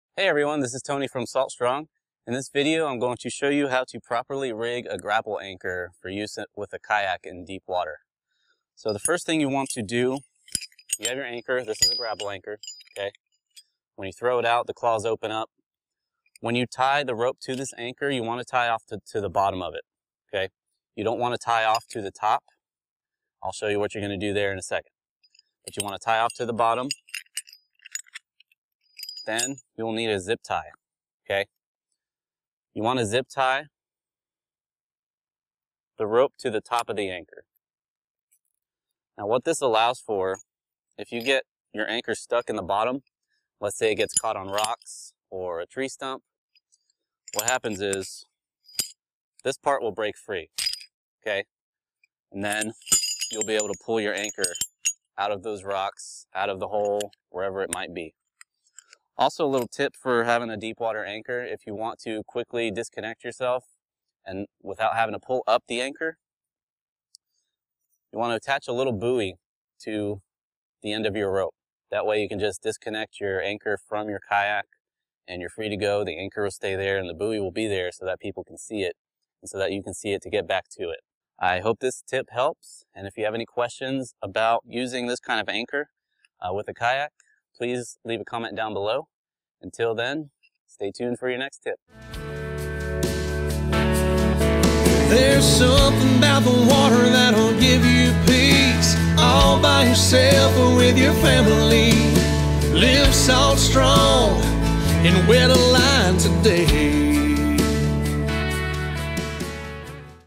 Kayak Grapnel Anchoring: How To Rig A Breakaway "Grapple" Anchor
Ever lost an anchor or had an anchor get really stuck to the bottom? It's not fun. And it's especially not fun if you are out in your kayak by yourself and you can't free your anchor.

One of the most popular kayak anchors is the grapnel anchor (pronounced Grapple Anchor). It is easy to use, it stores well, and "grapple anchors" don't cost very much.

We've recently noticed some kayak anglers rigging their grapnel anchors the wrong way (with the anchor line in the top hole). The best way to ensure you can always break the anchor free from the bottom is to rig it using the bottom ring combined with a zip-tie (or light monofilmanent fishing line) that can be broken if you are ever stuck.

The other trick to proper anchor management is having a buoy on board that you attach to your anchor line (that can quickly release from your kayak). This is ideal when you are fighting big fish that you need to chase down and don't have time to pull up your anchor (or want to keep your spot).

Watch Tony Acevedo from Salt Strong show how he quickly rigs this anchor up for a quick release.

Do you have any cool anchor tricks?